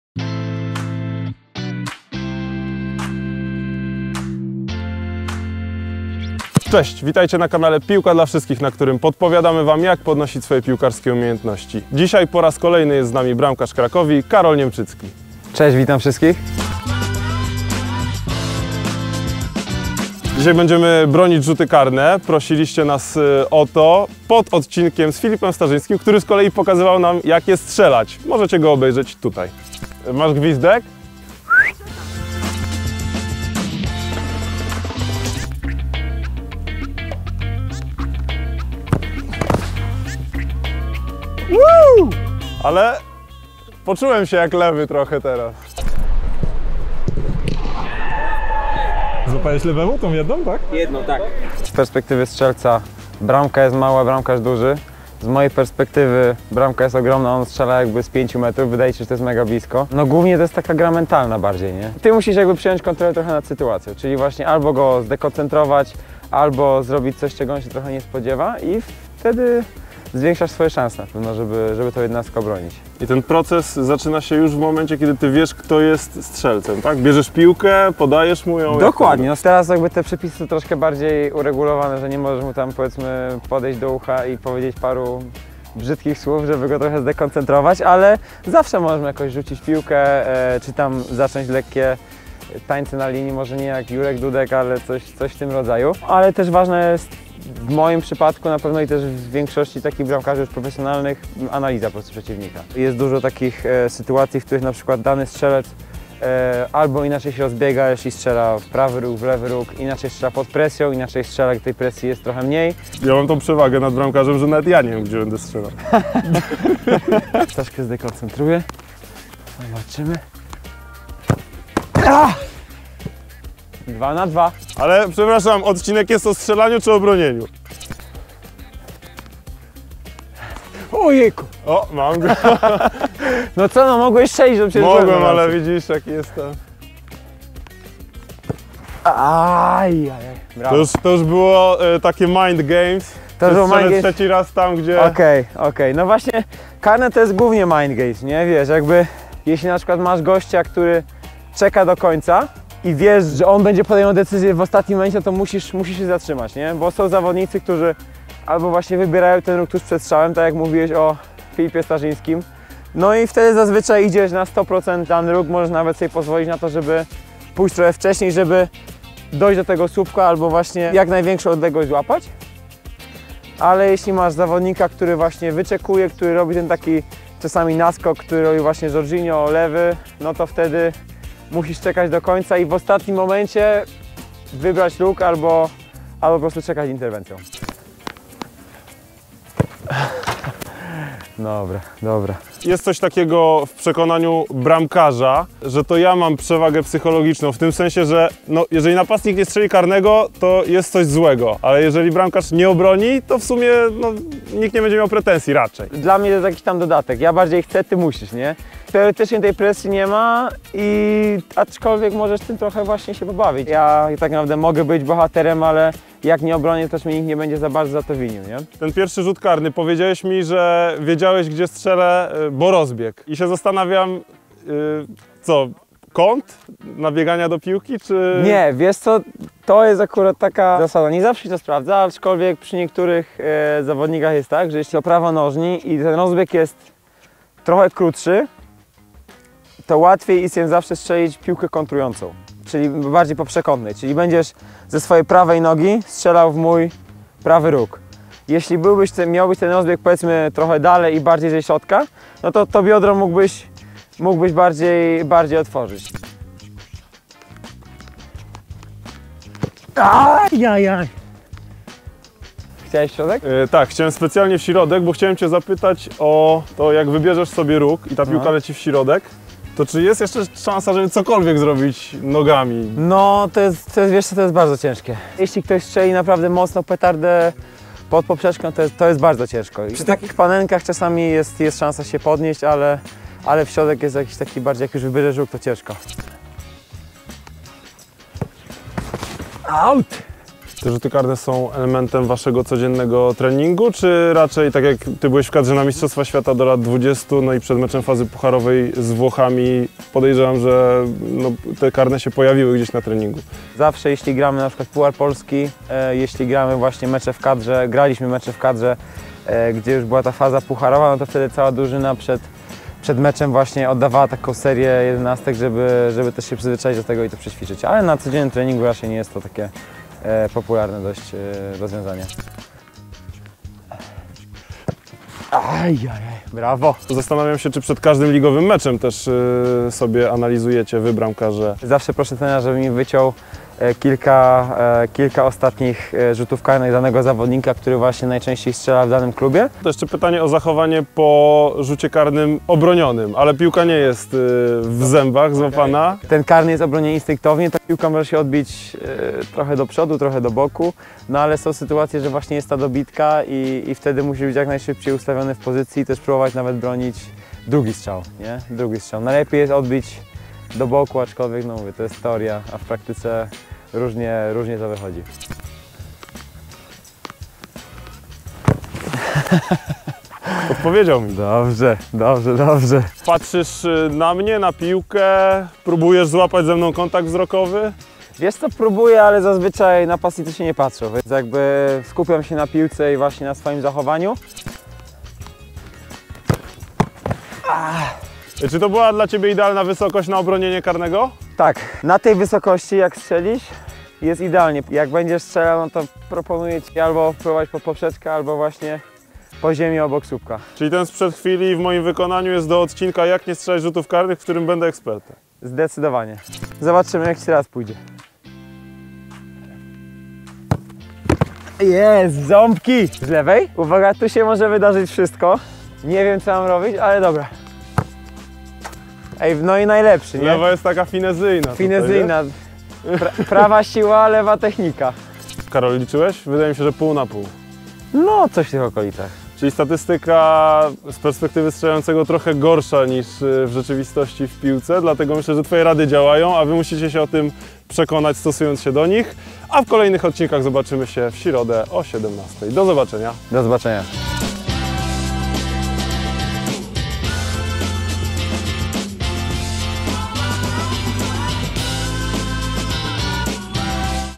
JAK BRONIĆ RZUTY KARNE? (ft. Karol Niemczycki | Cracovia)
Nie wiesz jak bronić rzuty karne? Jak się ustawić w bramce? Kiedy się rzucić? Na co zwrócić uwagę, gdy piłkarz wykonuje rzut karny? W jaki sposób broni rzuty karne bramkarz Cracovii, Karol Niemczycki?

Sprawdź nasz najnowszy odcinek, w którym bramkarz, który obronił rzut karny wykonywany przez Roberta Lewandowskiego, pokaże Ci, jak bronić rzuty karne!

Pamiętajcie o komentarzach!

Pozdrawiamy
ekipa Piłka dla wszystkich

Łączy nas piłka!